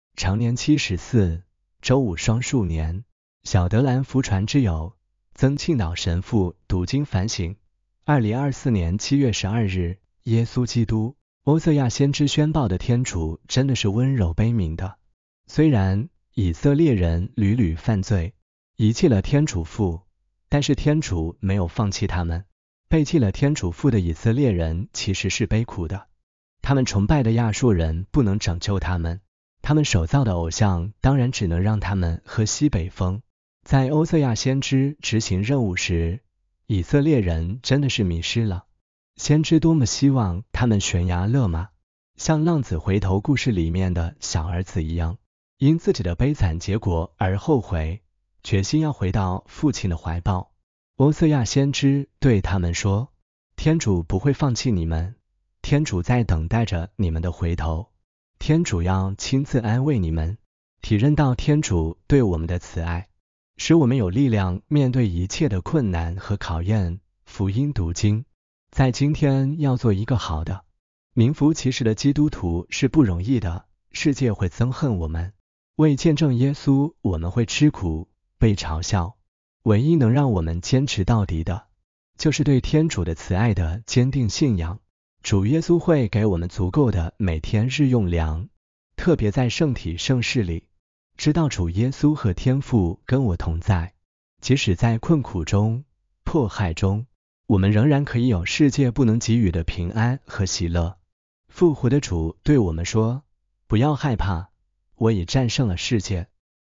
天主教#常年期14周五雙數年#小德蘭福傳之友#曾慶導神父#讀經反省#（2024.7.12）
常年期14周五雙數年

「 小德蘭福傳之友 」
曾慶導神父讀經反省（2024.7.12）

耶穌基督

歐瑟亞先知宣報的天主真的是溫柔悲憫的，雖然以色列人屢屢犯罪，遺棄了天主父，但是天主沒有放棄他們。背棄了天主父的以色列人其實是悲苦的，他們崇拜的亞述人不能拯救他們，他們手造的偶像當然只能讓他們喝西北風！在歐瑟亞先知執行任務時，以色列人真的是迷失了。先知多麼希望他們懸崖勒馬，像浪子回頭故事裡面的小兒子一樣，因自己的悲慘結果而後悔，決心要回到父親的懷抱。歐瑟亞先知對他們說：天主不會放棄你們，天主在等待著你們的回頭，天主要親自安慰你們！

體認到天主對我們的慈愛，使我們有力量面對一切的困難和考驗（福音讀經）。在今天要做一個好的，名符其實的基督徒是不容易的，世界會憎恨我們，為見證耶穌我們會吃苦,被嘲笑。唯一能讓我們堅持到底的，就是對天主的慈愛的堅定信仰。主耶穌會給我們足夠的每天日用糧，特別在聖體聖事裡。知道主耶穌和天父跟我同在，即使在困苦中，迫害中，我們仍然可以有世界不能給予的平安和喜樂。復活的主對我們說：＂不要害怕！我已戰勝了世界！＂